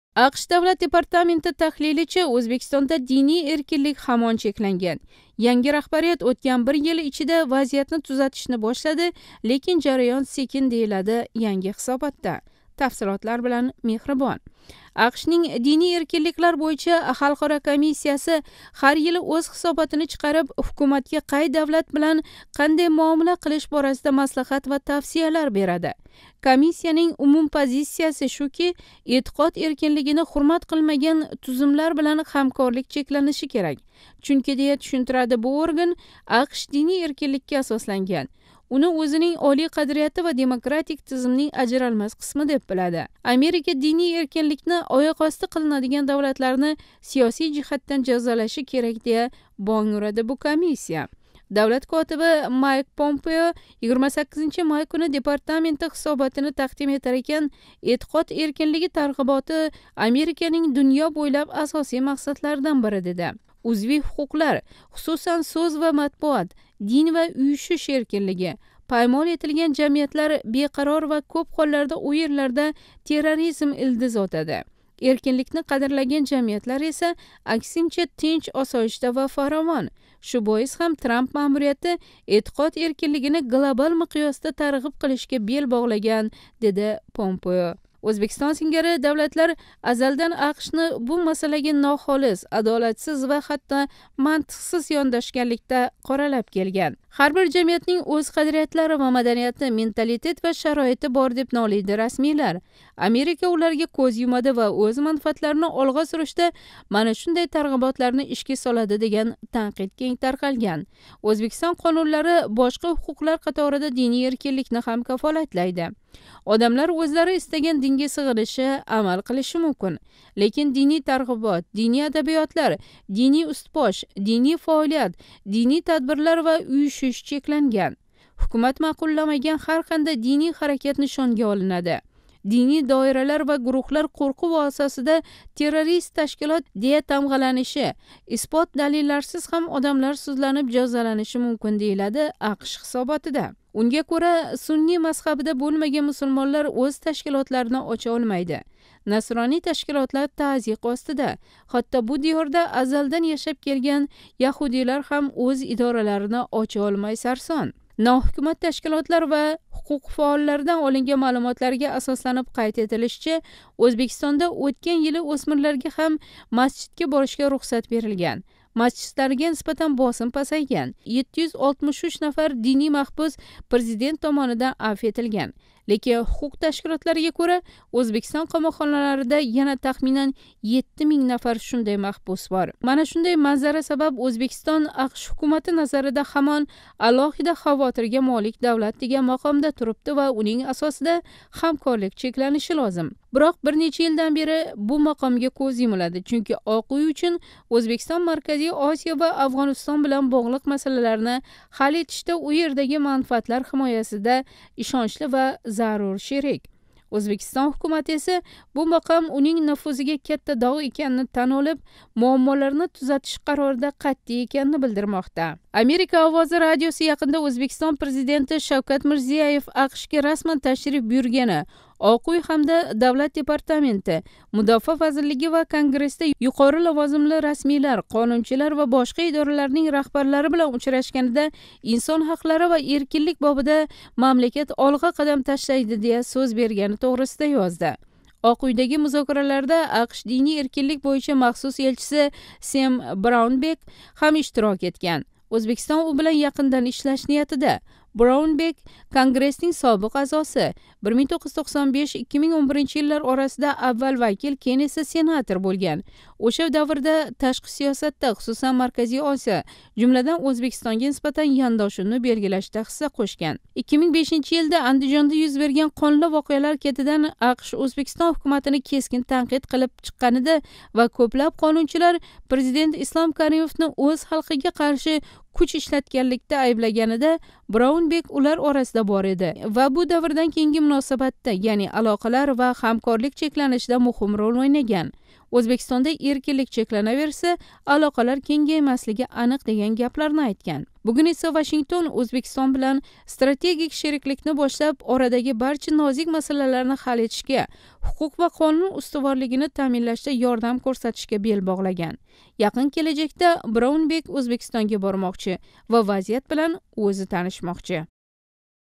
Ўзбекистон ҳамон эътиқод эркинлиги чекланган давлатлар қаторида
АҚШ Давлат департаменти таҳлилича, Ўзбекистонда диний эркинлик ҳамон чекланган. Янги раҳбарият ўтган бир ярим йил ичида вазиятни тузатишни бошлади, лекин жараён секин, дейилади янги ҳисоботда.

АҚШнинг Диний эркинликлар бўйича Халқаро комиссияси ҳар йили ўз ҳисоботини чиқариб, ҳукуматга қай давлат билан қандай муомала қилиш борасида маслаҳат ва тавсиялар беради. Комиссиянинг умум позицияси шуки, эътиқод эркинлигини ҳурмат қилмаган тузумлар билан ҳамкорлик чекланиши керак. Чунки, дея тушунтиради бу орган, АҚШ диний эркинликка асосланган, уни ўзининг олий қадрияти ва демократик тизимнинг ажралмас қисми, деб билади. Америка диний эркинликни оёқ ости қилинадиган давлатларни сиёсий жиҳатдан жазолаши керак, дея бонг уради бу комиссия.

Давлат котиби Майк Помпео 28-май куни департаменти ҳисоботини тақдим этар экан, эътиқод эркинлиги тарғиботи Американинг дунё бўйлаб асосий мақсадларидан бири, деди.

«Узвий ҳуқуқлар, хусусан сўз ва матбуот, дин ва уюшиш эркинлиги, поймол этилган жамиятлар беқарор ва кўп ҳолларда у ерларда терроризм илдиз отади. Эркинликни қадрлаган жамиятлар эса аксинча, тинч, осойишта ва фаровон. Шу боис ҳам Трамп маъмурияти эътиқод эркинлигини глобал миқёсда тарғиб қилишга бел боғлаган», - деди Помпео.